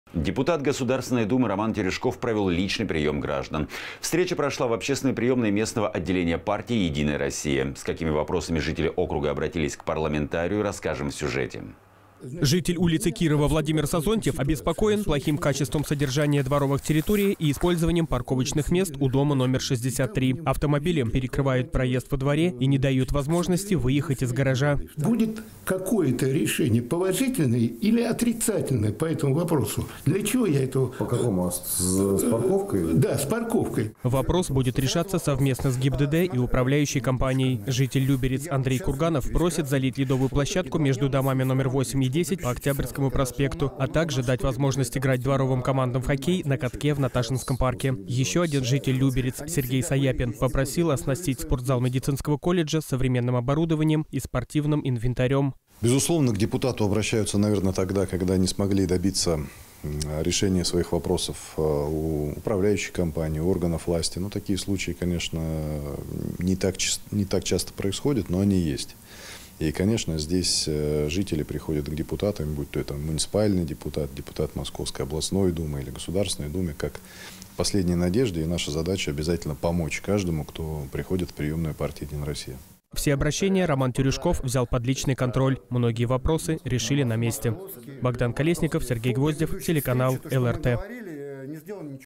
Личный приём депутата Госдумы Романа Терюшкова
Депутат Государственной Думы Роман Терюшков провёл личный приём граждан. Встреча прошла в общественной приёмной местного отделения партии «Единая Россия». С какими вопросами жители округа обратились к парламентарию, расскажем в сюжете.

Житель улицы Кирова Владимир Сазонтьев обеспокоен плохим качеством содержания дворовой территории и использованием парковочных мест у дома № 63. Автомобили перекрывают проезд во дворе и не дают возможности выехать из гаража.

Вопрос будет решаться совместно с ГИБДД и управляющей компанией. Житель Люберец Андрей Курганов просит залить ледовую площадку между домами №№ 8 и 10 по Октябрьскому проспекту, а также дать возможность играть дворовым командам в хоккей на катке в Наташинском парке. Ещё один житель Люберец Сергей Саяпин попросил оснастить спортзал медицинского колледжа современным оборудованием и спортивным инвентарём.

Роман Терюшков, депутат Государственной Думы РФ: «Безусловно, к депутату обращаются, наверное, тогда, когда не смогли добиться решения своих вопросов у управляющих компаний, органов власти. Такие случаи, конечно, не так часто происходят, но они есть. И, конечно, здесь жители приходят к депутатам, будь то муниципальные депутаты, депутаты Московской областной Думы или Государственной Думы как к последней надежде. И наша задача обязательно помочь каждому, кто приходит в приёмную партии «Единая Россия»».

Все обращения Роман Терюшков взял под личный контроль. Многие вопросы решили на месте.